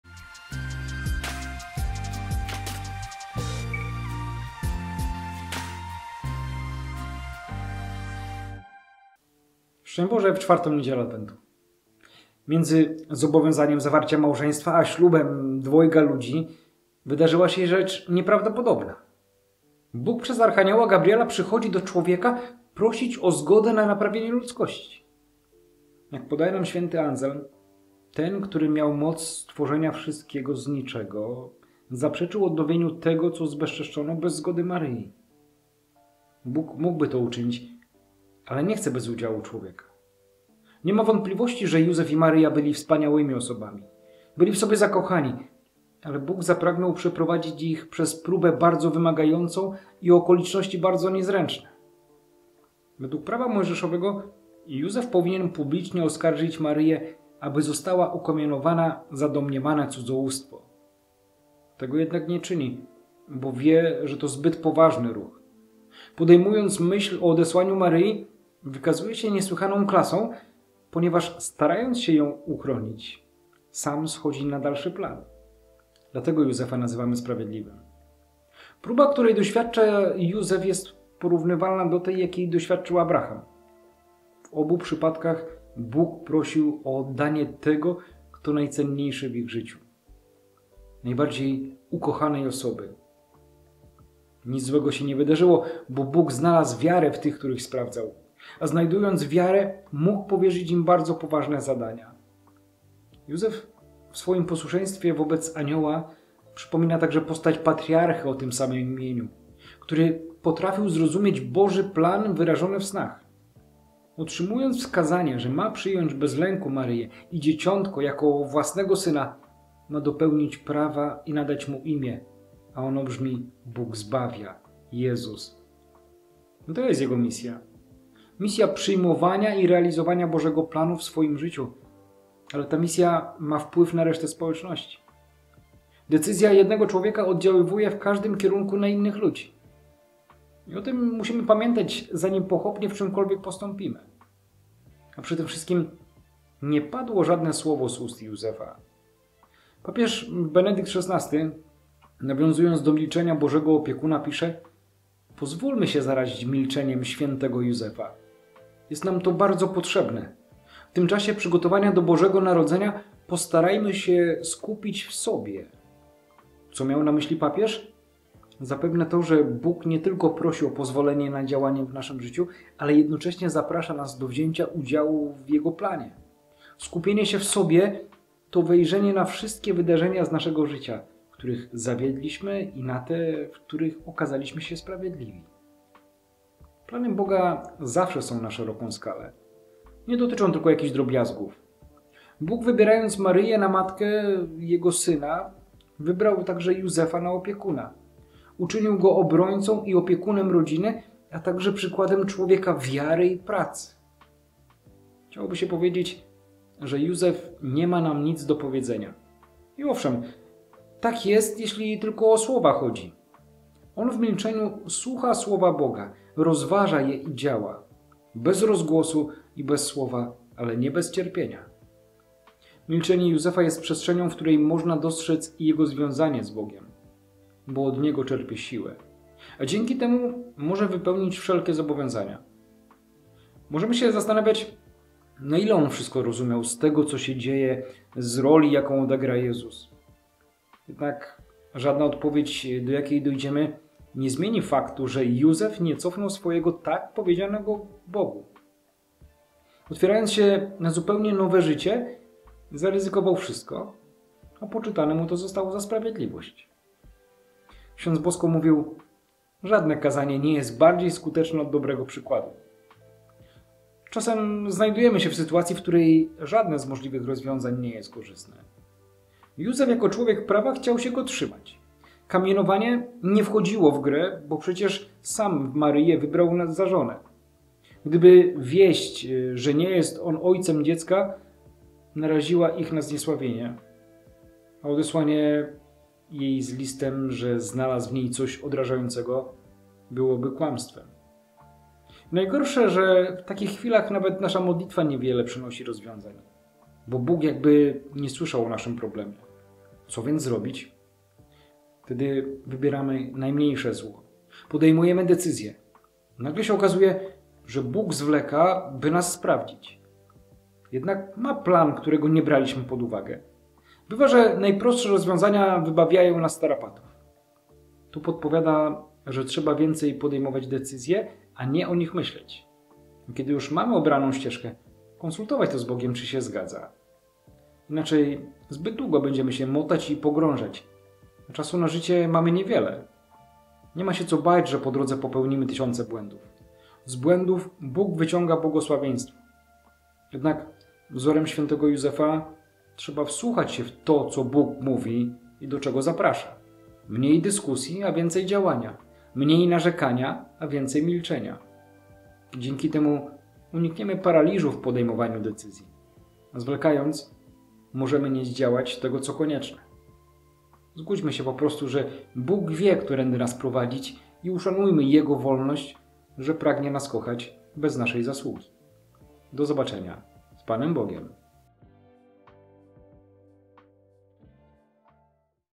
4 Niedziela Adwentu rok A (Mt 1, 18-24) swiatloslowa IV niedziela adwentu a
4 Niedziela Adwentu rok A (Mt 1, 18-24) swiatloslowa danielgeppert (IV niedziela adwentu)
Próba, której doświadcza Józef jest porównywalna do tej, jakiej doświadczył Abraham. W obu przypadkach Bóg prosił o oddanie tego, kto najcenniejszy w ich życiu. Najbardziej ukochanej osoby. Nic złego się nie wydarzyło, bo Bóg znalazł wiarę w tych, których sprawdzał. A znajdując wiarę mógł powierzyć im bardzo poważne zadania.

4 niedziela adwentu rok a  Mt 1, 18-24
IV Niedziela Adwentu rok A (Mt 1, 18-24)